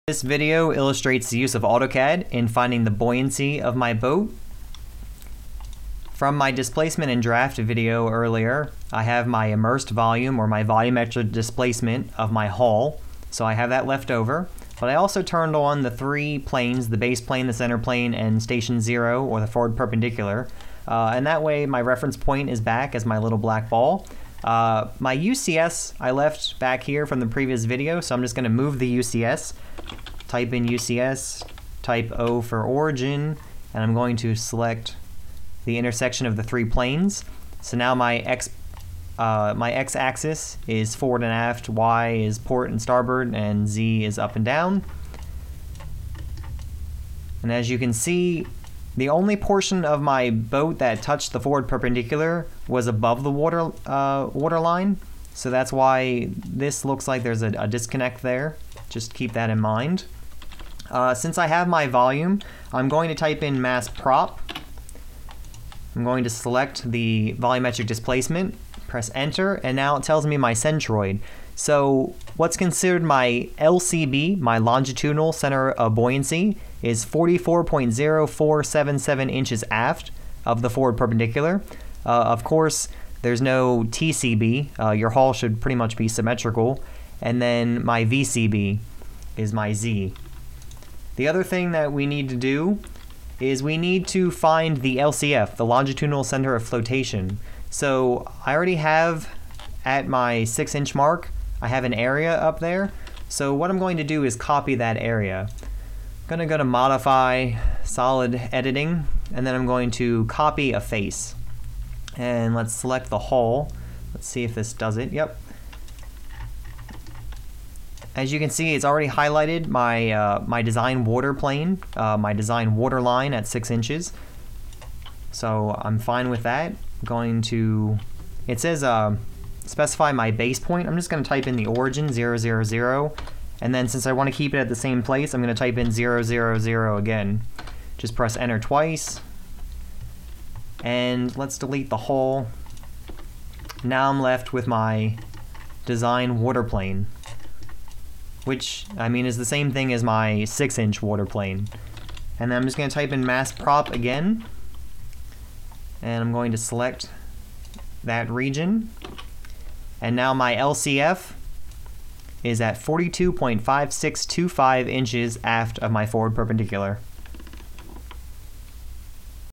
AutoCAD Tutorial Video - Buoyancy
AutoCAD tutorial video for buoyancy calculations for the the Boat Design Competition, an annual steel boat competition for high school students hosted by The Apprentice School SNAME chapter of Newport News Shipbuilding